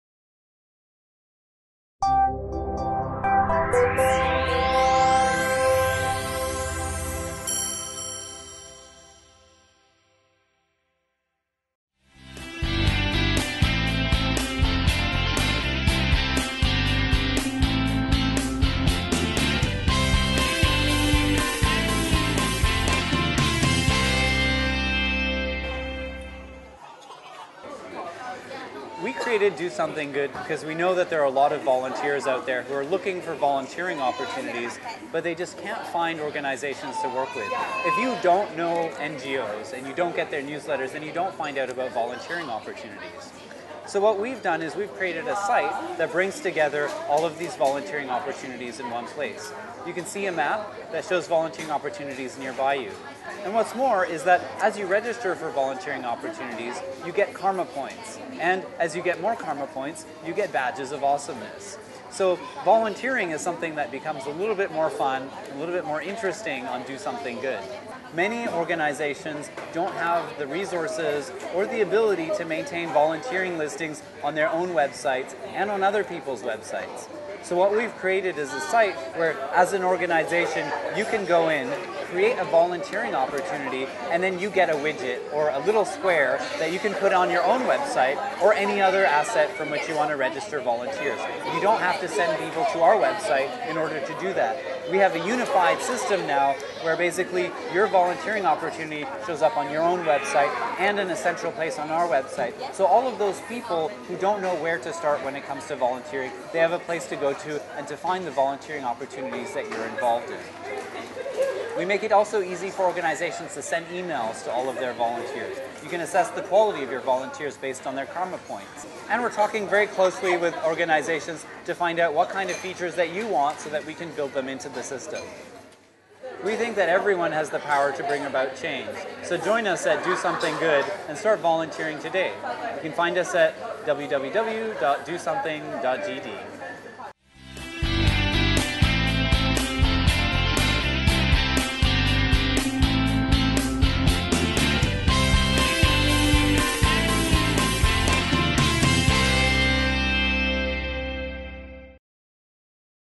Do Something Good
We met up with the folks for Do Something Good last saturday and here's what Kal Joffres of Do Something Good had to say about the online volunteering platform.